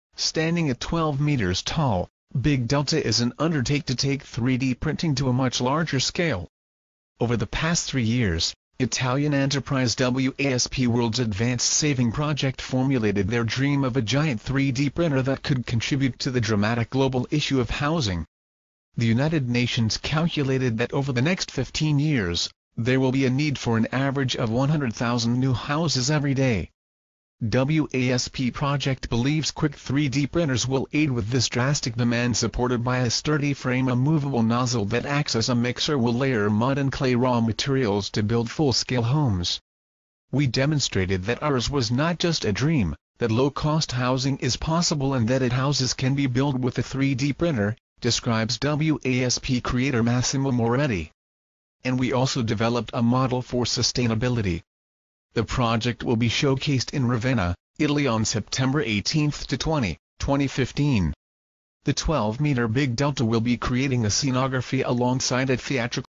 large-scale 3D printer plans to build homes using organic materials
standing at 12 meters tall, ‘big delta’ is an undertake to take 3D printing to a much larger scale. over the past three years, italian enterprise WASP (world’s advanced saving project) formulated their dream of a giant 3D printer that could contribute to the dramatic global issue of housing. the united nations calculated that over the next 15 years, there will be a need for an average of 100,000 new houses every day. WASP project believes quick 3D printers will aid with this drastic demand. supported by a sturdy frame a movable nozzle that acts as a mixer will layer mud and clay raw materials to build full-scale homes.
‘we demonstrated that ours was not just a dream, that low-cost housing is possible and that it houses can be built with a 3D printer’, describes WASP creator massimo moretti. ‘and we also developed a model for sustainability.’ the project will be showcased in ravenna, italy on september 18 to 20, 2015. the 12 meter ‘big delta’ will be creating a scenography alongside a theatrical piece.